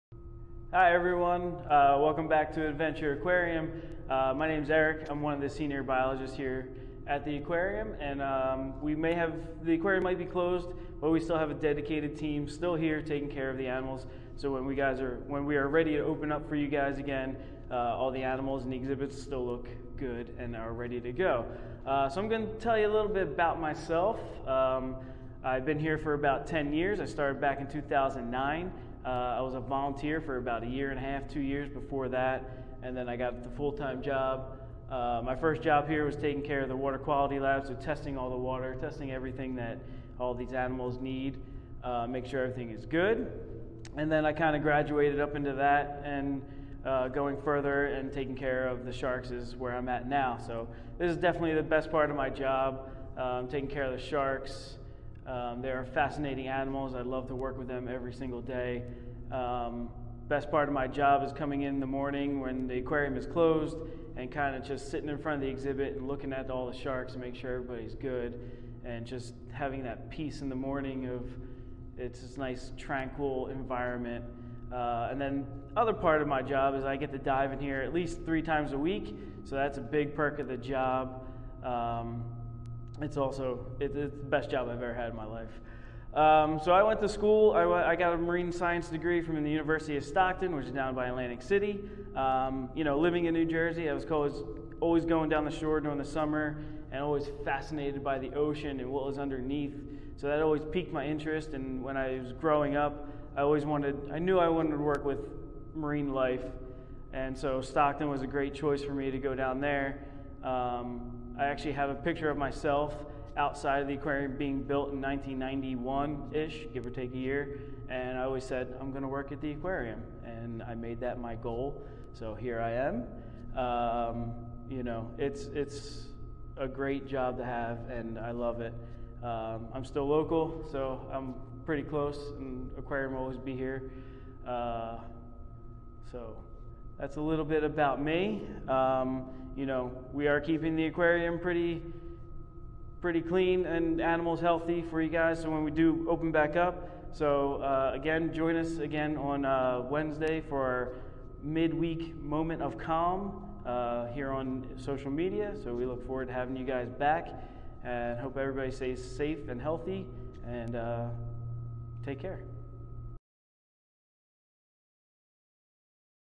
Hard at Work with Erik, Senior Biologist and Shark Caretaker
Learn more about what it's like to take care of sharks for a living.